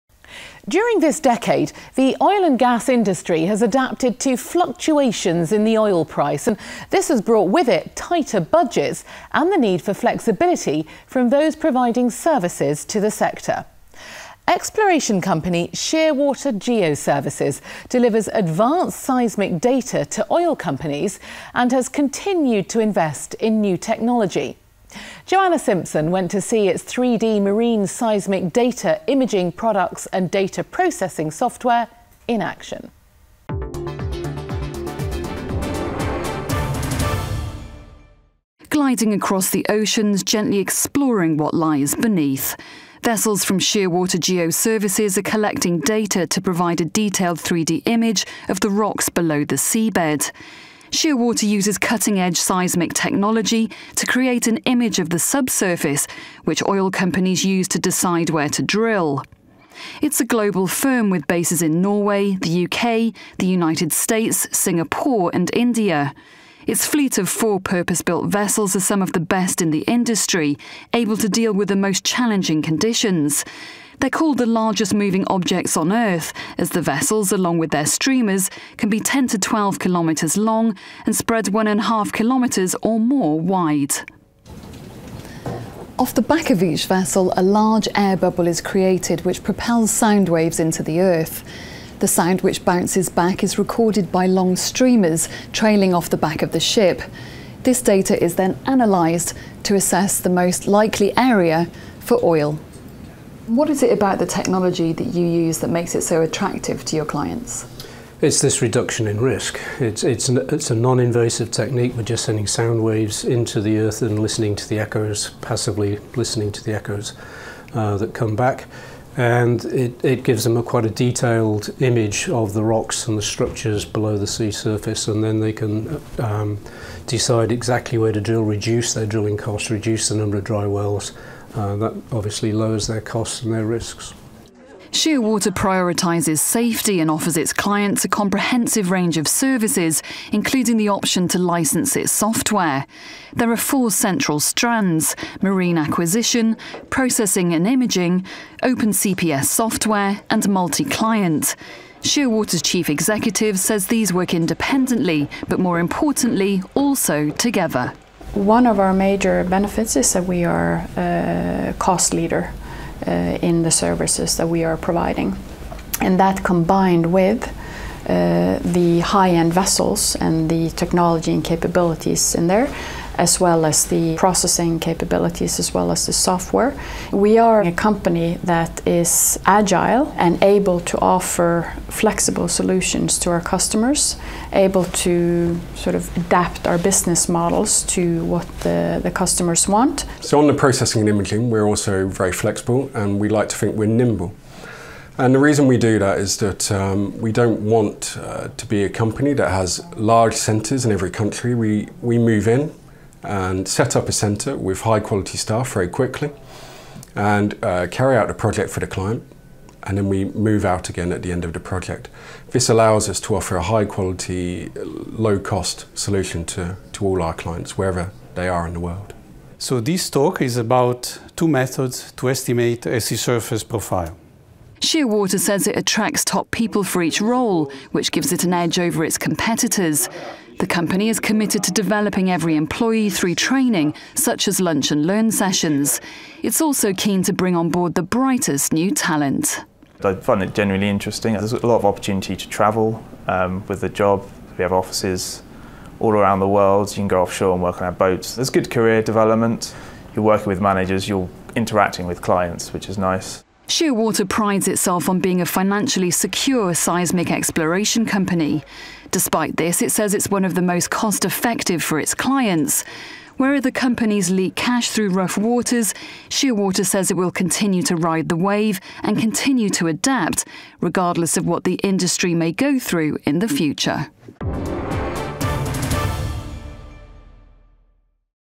Continued investment in new technology with Shearwater GeoServices
How using cutting edge seismic technology can help oil companies decide where to drill.